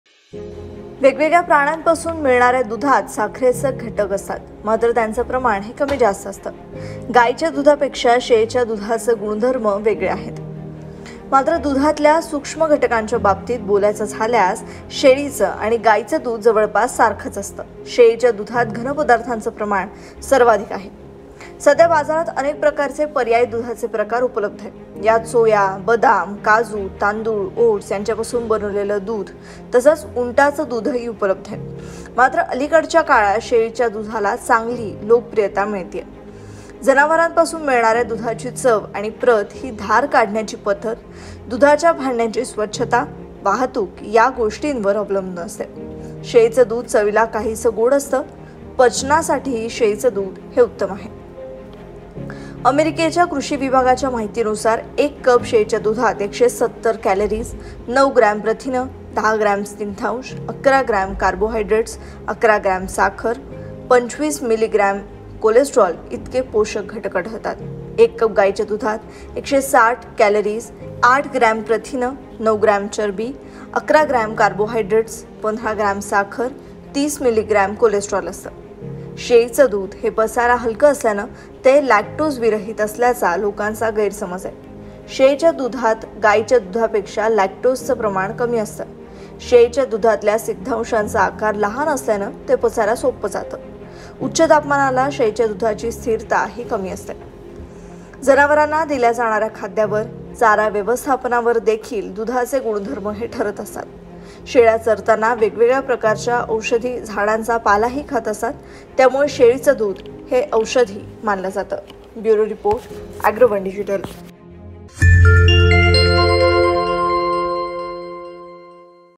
गायीच्या आणि शेळीच्या दुधातला फरक | Goat Milk Vs Cow Milk | ॲग्रोवन
शेळीचे_दूध आरोग्यास लाभकारी असते. शेळीच्या दुधाचे महत्त्व काय आहे ? शेळीचे दूध गायीच्या दुधापासून भिन्न कसे हे जाणून घेण्यासाठी हा व्हिडिओ पाहा.
To know difference between goat_milk and cow_milk, watch the video.

Characteristics of cow

Characteristics of goat milk

आमच्याशी जोडून राहण्यासाठी भेट द्या